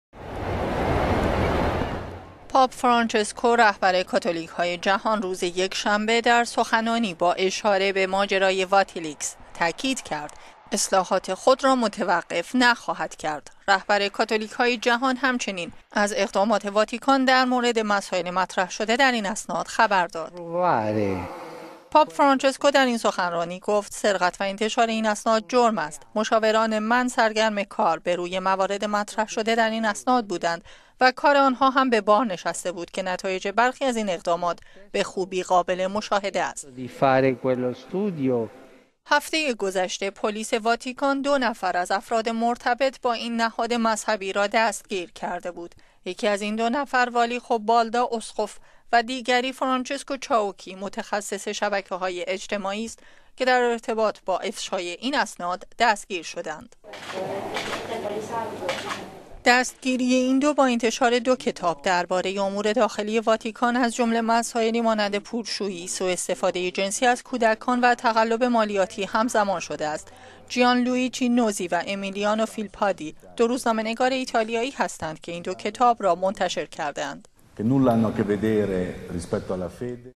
پاپ: اصلاحات در واتیکان متوقف نخواهد شد
پاپ فرانچسکو، رهبر کاتولیک های جهان روز یکشنبه در سخنانی با اشاره به ماجرای معروف به «واتیلیکس» تاکید کرد اصلاحات خود را متوقف نخواهد کرد. وی با بیان این مطلب در سخنرانی عمومی هفتگی خود در مقابل هزاران نفر از مردم در میدان سنت پی یر، افشای این اسناد را جرم خواند.

این نخستین بار است که پاپ به طور عمومی و رسمی دربارۀ افشای اسناد مربوط به اسرار واتیکان اظهار نظر می کند. رهبر کاتولیک های جهان همچنین از اقدامات واتیکان در مورد مسائل مطرح شده در این اسناد خبر داد.